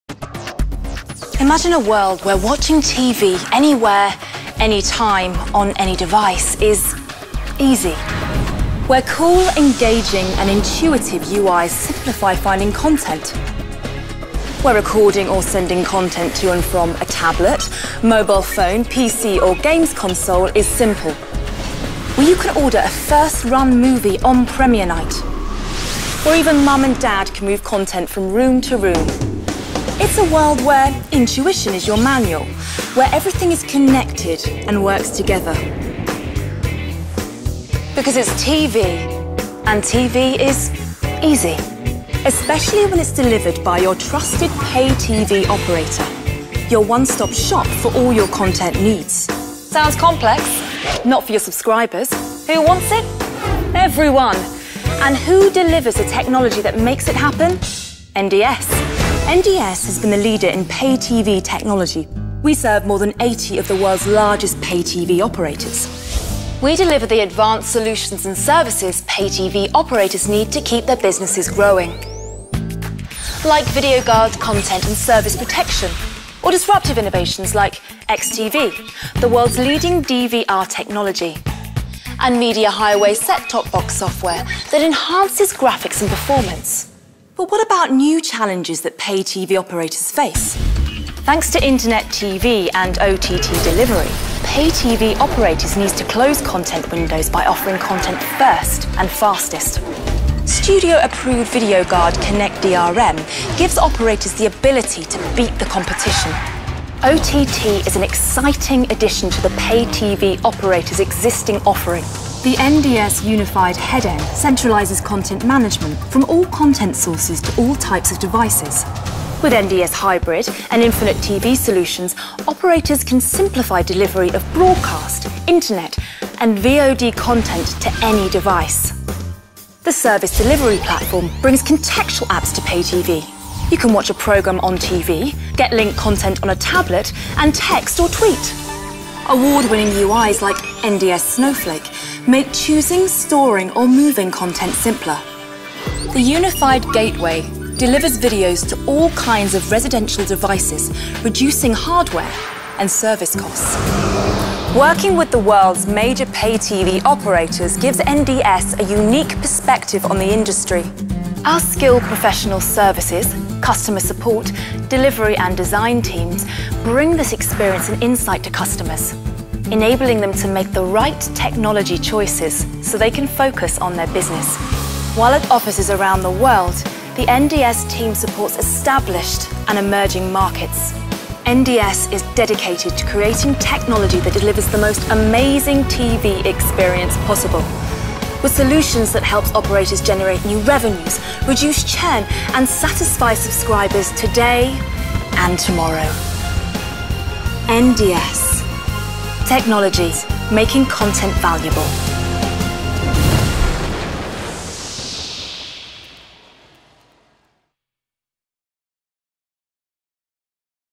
NDS - Corporate Promotional Film 2011
NDS is a leading global provider of end-to-end software solutions for the pay television industry. We enable pay-TV operators to differentiate their service by delivering a unique viewing experience to their subscribers—anytime, anywhere on any device—while ensuring that only paying viewers can view content.

Over one-third of the world's digital pay-TV households rely on our technologies to discover, navigate or interact with content. Our clients include many of the largest cable, satellite and broadband pay-TV operators, including Astro, Bharti, BSkyB, Canal Plus, China Central Television ("CCTV"), Cox, DIRECTV, Kabel Deutschland, Sky Deutschland, Sky Italia, TataSky, UPC (a unit of Liberty Global) and Vodafone. NDS is very active in emerging markets where the move from analog to digital is now occurring.